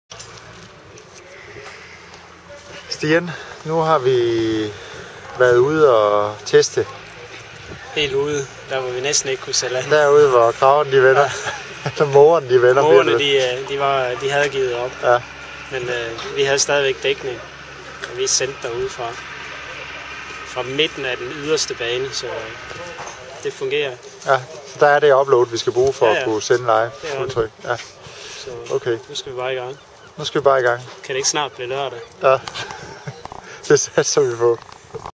Sportcasting 2.0, streaming,TORM Ungdoms DM, test evaluering
Dette er Stian Eide fra StreamFactory, som evaluerer test af streaming fra Århus Bugten forud for live streaming af TORM Ungdoms- og Junior DM.  Distributed by Tubemogul.